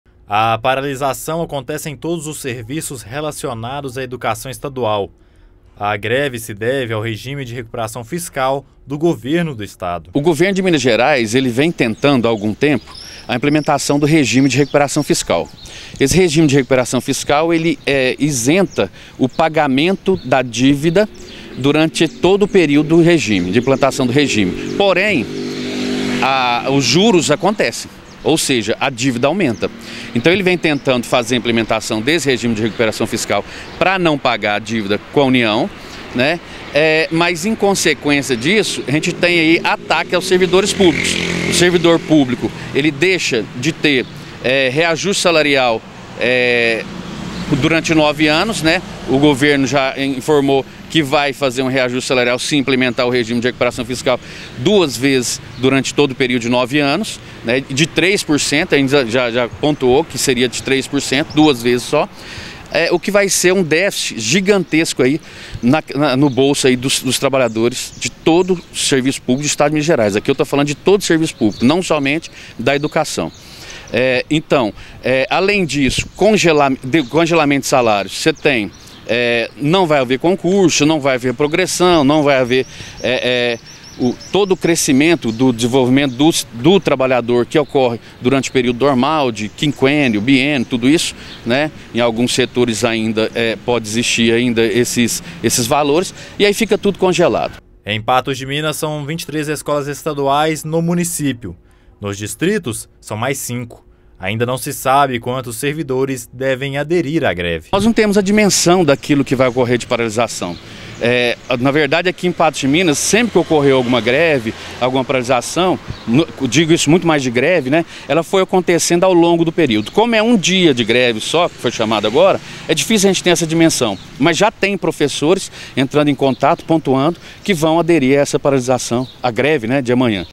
Alerta de greve em escolas estaduais
Greve na Educação em Minas Gerais, regime de recuperação fiscal, impacto nos salários dos servidores públicos - A comunidade de Patos de Minas está diante de um movimento grevista que desafia as políticas estaduais. Professores e funcionários das escolas estaduais se posicionam contra as medidas de austeridade que congelam salários e suspendem progressões. A NTV Patos de Minas traz cobertura completa, destacando as vozes dos educadores e as repercussões para a população.

A greve dos servidores da educação em Minas Gerais, iniciada em resposta ao regime de recuperação fiscal proposto pelo governo estadual, coloca em destaque o delicado equilíbrio entre a gestão responsável das finanças públicas e a manutenção dos direitos dos funcionários públicos. A proposta, que prevê a suspensão do pagamento da dívida do estado em troca de um congelamento salarial extenso e a suspensão de concursos e progressões de carreira, tem sido motivo de grande controvérsia. Profissionais da educação de Patos de Minas e adjacências, já afetados por anos sem reajustes significativos, veem-se agora diante de uma perspectiva de quase uma década sem aumento real nos salários, o que levanta sérias preocupações sobre a valorização de sua profissão e as repercussões na qualidade do ensino oferecido à população. A NTV Patos de Minas mergulha nesta discussão, trazendo à tona as vozes dos educadores, especialistas e autoridades para um debate aprofundado sobre as implicações deste regime fiscal. A emissora reforça a necessidade urgente de um diálogo aberto e efetivo entre as partes envolvidas, buscando soluções que equilibrem as demandas fiscais com o imperativo de investir no capital humano, essencial para o desenvolvimento socioeconômico do estado. Através de uma cobertura jornalística criteriosa e engajada, a NTV se estabelece como um canal de informação essencial, promovendo a conscientização e fomentando a participação cívica neste debate crucial.

A cobertura da NTV sobre a greve dos servidores da educação em Minas Gerais proporcionou vários aprendizados. Primeiramente, destacou-se a compreensão do regime de recuperação fiscal e suas implicações para os servidores públicos, incluindo o congelamento de salários e a suspensão de concursos e progressões de carreira. Foi evidenciado o impacto direto dessas medidas na vida dos educadores, que enfrentam a perspectiva de nove anos sem reajustes salariais significativos. Além disso, a reportagem iluminou a complexidade das negociações entre sindicatos e o governo, e a importância da manutenção do diálogo para encontrar soluções que atendam tanto às necessidades fiscais do estado quanto aos direitos dos trabalhadores. Aprendeu-se também sobre a resposta da comunidade educacional, com professores expressando sua disposição para aderir à greve, refletindo o descontentamento com as propostas atuais. Por fim, a cobertura reforçou a importância do jornalismo local na articulação de debates públicos sobre questões que afetam diretamente a população, demonstrando o papel da NTV como um veículo de informação e discussão para a comunidade de Patos de Minas e região.

O que motivou a greve dos servidores da educação em Minas Gerais?
A greve foi motivada pela implementação do regime de recuperação fiscal pelo governo de Minas Gerais, que implica o congelamento de salários dos servidores públicos e a suspensão de concursos e progressões de carreira por um período de nove anos.

Quais são as consequências do regime de recuperação fiscal para os servidores públicos?
As consequências incluem a ausência de reajustes salariais significativos durante nove anos, com apenas dois aumentos previstos de 3%, além do congelamento de concursos, progressões e outros benefícios relacionados ao desenvolvimento e crescimento profissional dos servidores.

Quantas escolas estaduais existem em Patos de Minas e nos distritos? Em Patos de Minas, existem 23 escolas estaduais e mais cinco nos distritos.

Qual é a expectativa de adesão à greve em Patos de Minas?
Não há uma dimensão clara de quantos servidores vão aderir à greve, mas já há professores entrando em contato e pontuando que vão participar da paralisação.